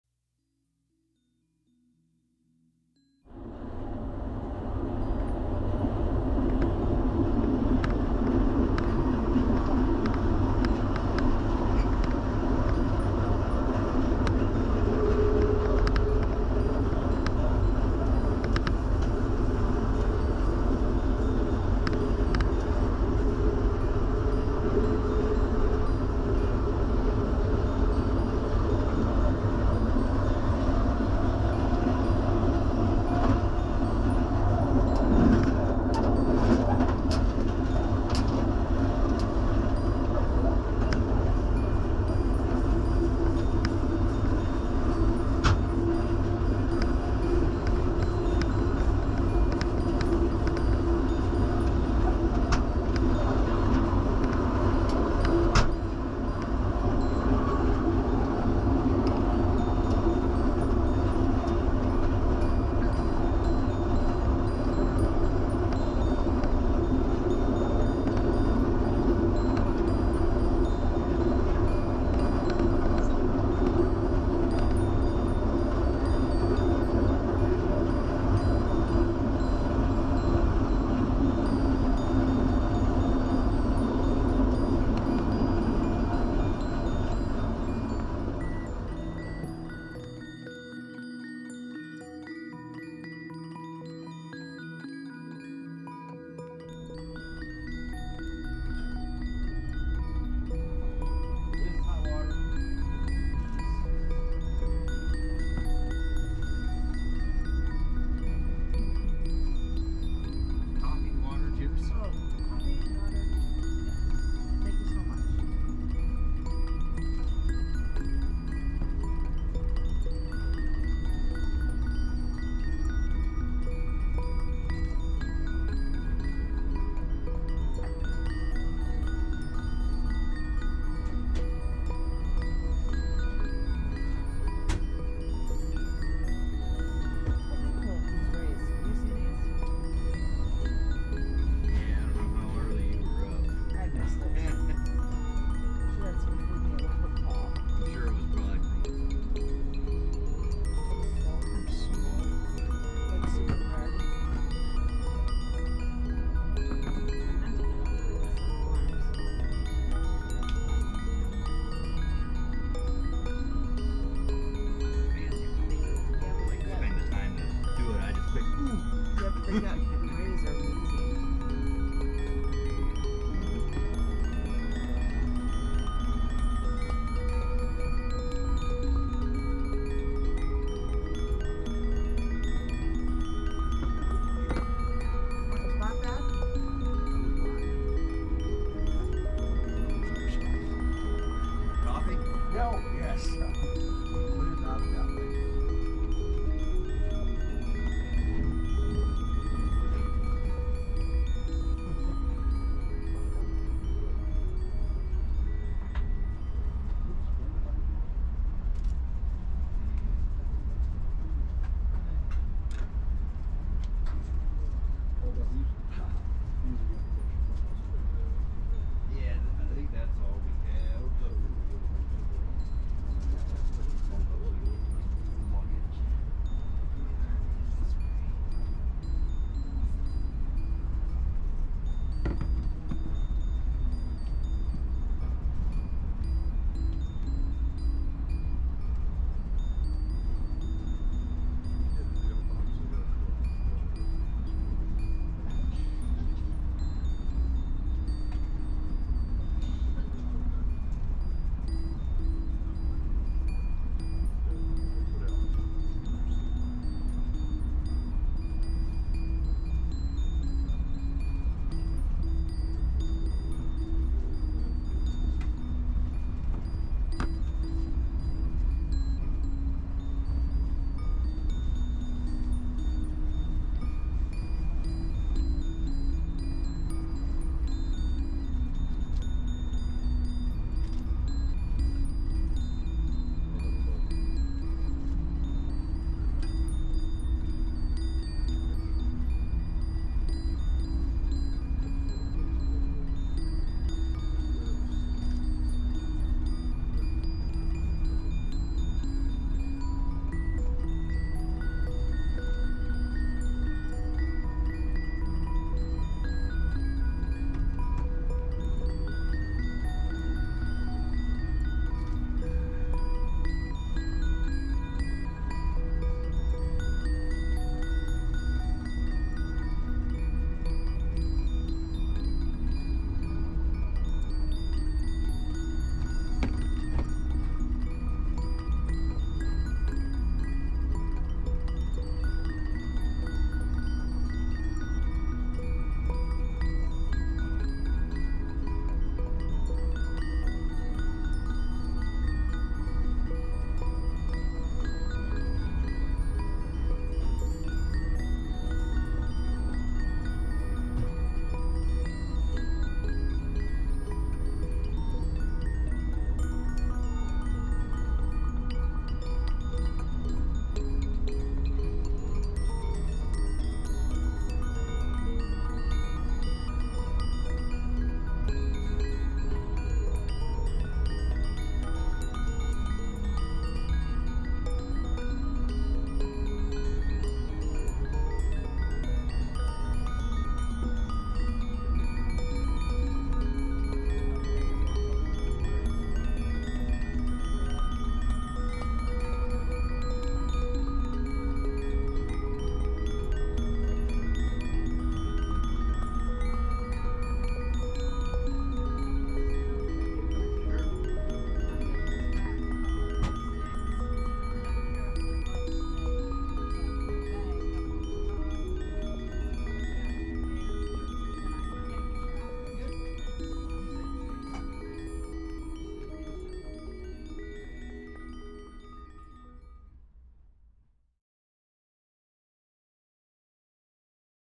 Private rail cars behind Amtrak California Zephyr dome car sunrise and sunset Utah salt flats
Riding Superdome 53, Pacific Union, and Cimarron River behind California Zephyr.